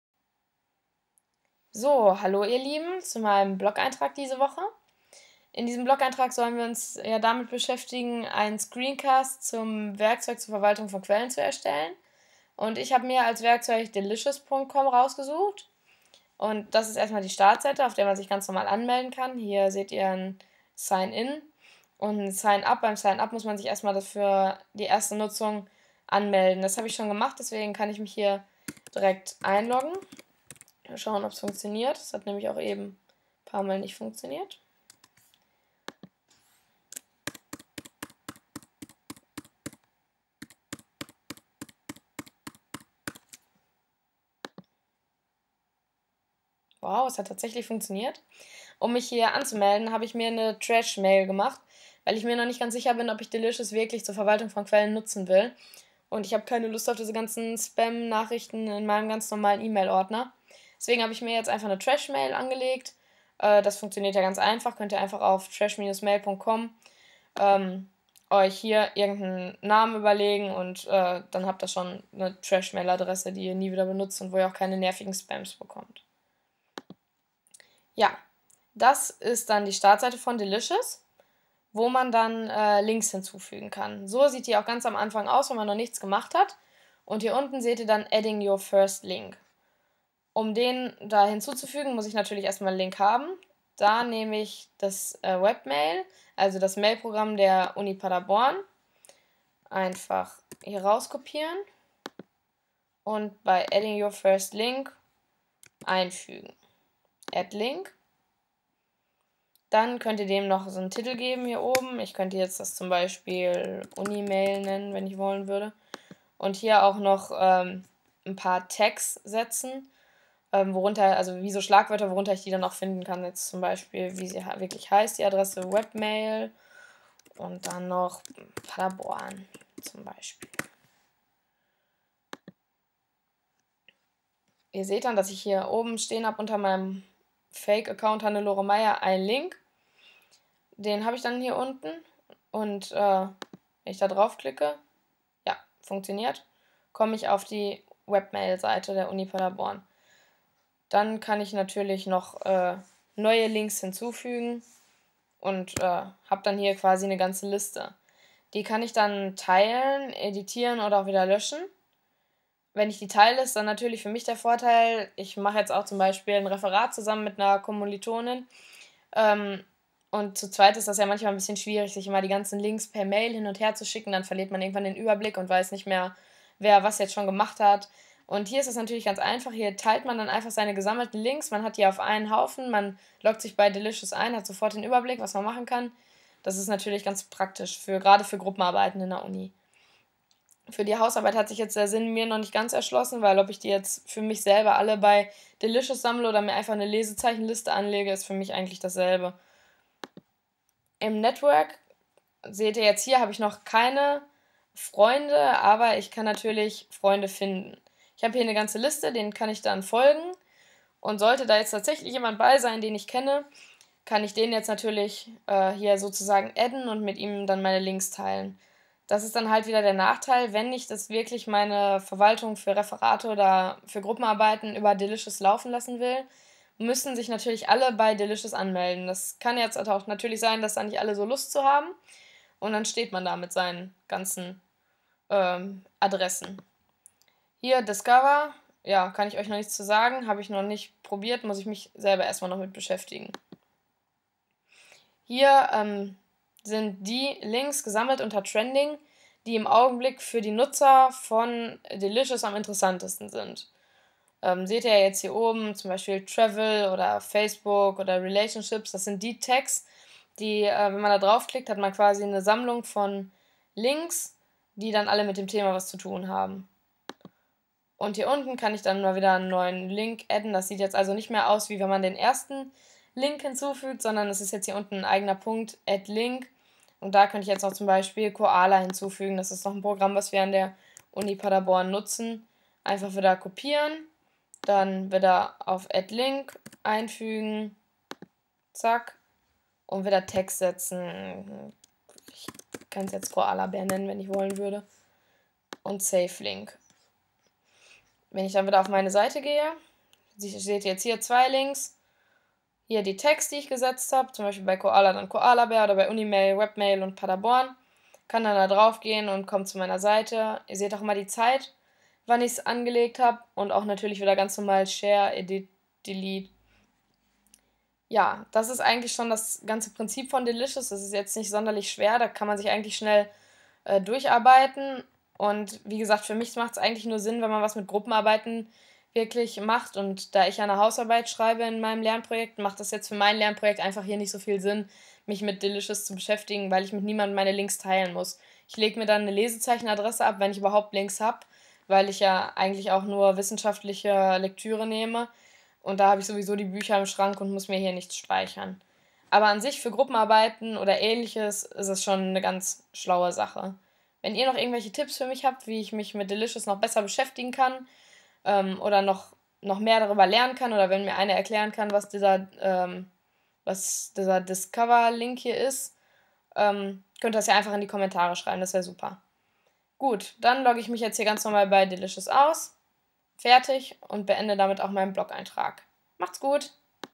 Delicious - ein Screencast
Hier findet ihr meinen Blogbeitrag zum Thema "Werkzeuge zur Literaturverwaltung" - ein Screencast für das Seminar "eStudi".

Liebe Grüße, Sabine